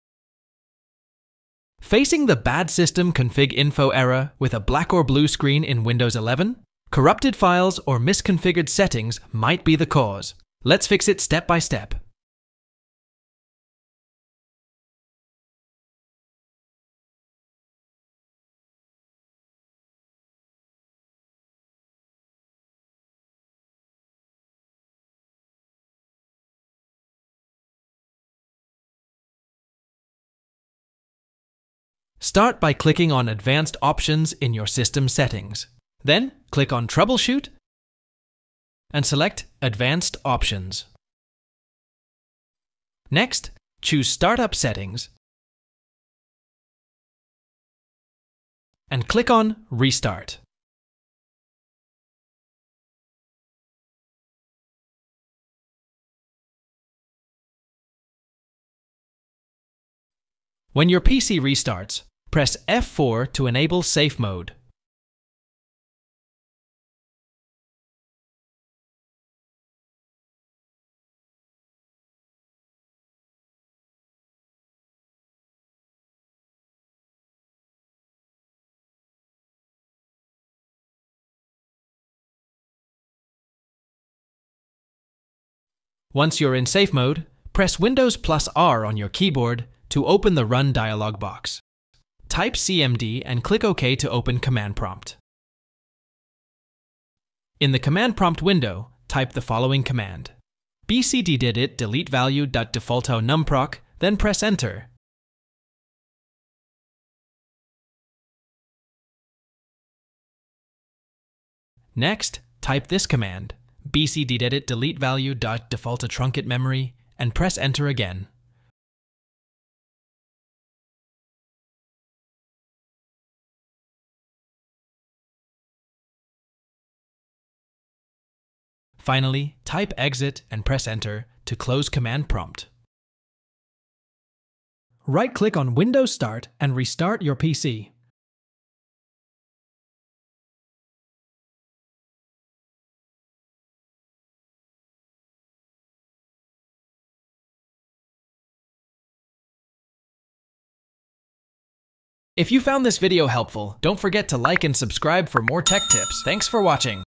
BAD SYSTEM CONFIG INFO With Black or Blue Screen in Windows 11 / 10 / 8 / 7 - How To Fix Error ✅
Are you encountering the BAD SYSTEM CONFIG INFO error with a black or blue screen in Windows 11? This frustrating issue often arises due to corrupted system files, incorrect Boot Configuration Data (BCD), or misconfigured settings. In this video, we'll guide you through effective solutions step-by-step to fix this error and get your system back to normal. Watch now to resolve this issue and improve your Windows 11 experience!

00:00 Intro
00:14 System settings
01:27 cmd in safe mode
02:32 Outro

Command Used:
bcdedit/deletevalue {default} numproc
bcdedit/deletevalue {default} truncatememory